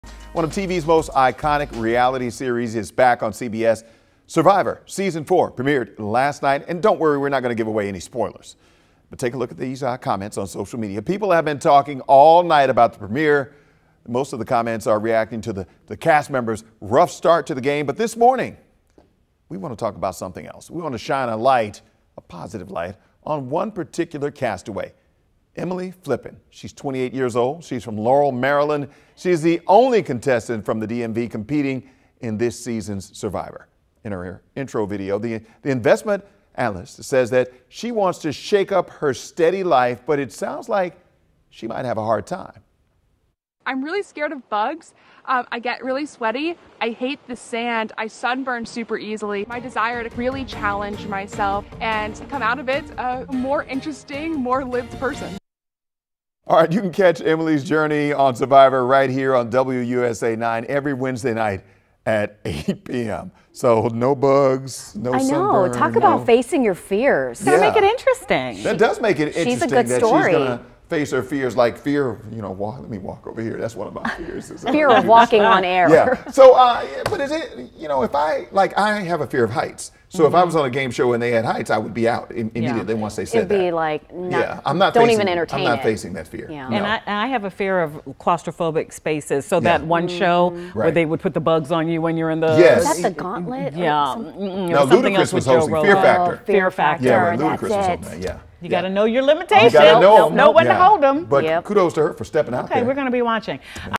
Laurel resident competes on new season of 'Survivor'
Emily Flippen is the only contestant from the DMV